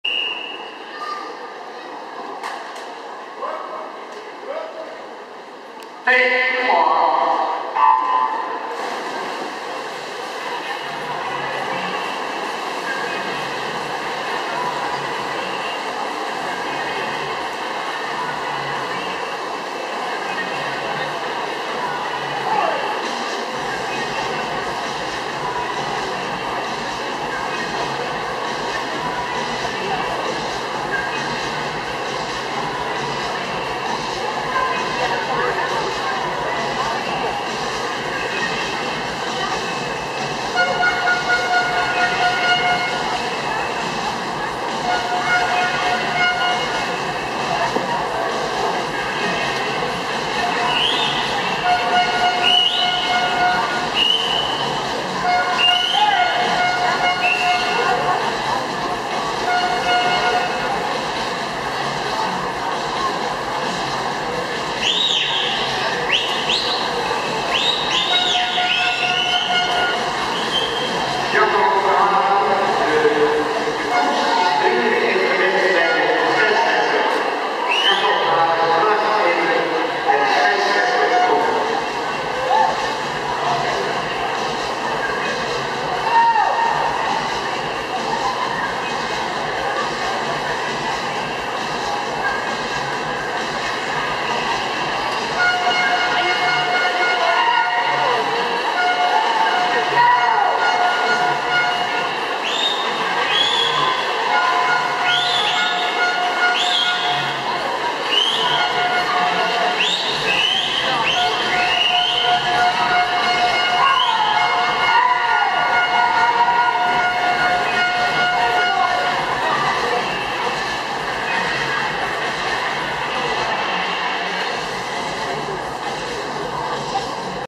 B-Finale 200m vrije slag Swim Cup Eindhoven 2012
Finale B
9. Emmanuel Vanluchene Gold Swimming Team 1:52.89 GOLD/011/92 1:51.26 +0,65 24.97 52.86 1:22.13 1:51.26
10. Marawan Aly Egyptian Team 1:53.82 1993MaAl 1:52.43 +0,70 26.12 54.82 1:23.81 1:52.43
11. Jurjen Willemsen EIFFELswimmersPSV 1:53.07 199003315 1:52.80 +0,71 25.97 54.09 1:23.46 1:52.80
12. Simon Rabold Schwimmclub Uster Wallisellen 1:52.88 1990SiRa 1:52.93 +0,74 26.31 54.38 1:23.50 1:52.93
13. Steven Nonnekes EIFFELswimmersPSV 1:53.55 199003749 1:53.51 +0,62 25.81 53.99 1:23.33 1:53.51
14. Richard Nagy J&T Sport Team 1:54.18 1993RiNa 1:54.38 +0,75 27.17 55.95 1:25.60 1:54.38
15. Bo Wullings De Dolfijn 1:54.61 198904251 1:55.51 +0,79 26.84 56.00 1:25.98 1:55.51
16. Lucien Haßdenteufel SSg Saar Max Ritter 1:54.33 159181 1:58.50 +0,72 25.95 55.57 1:26.75 1:58.50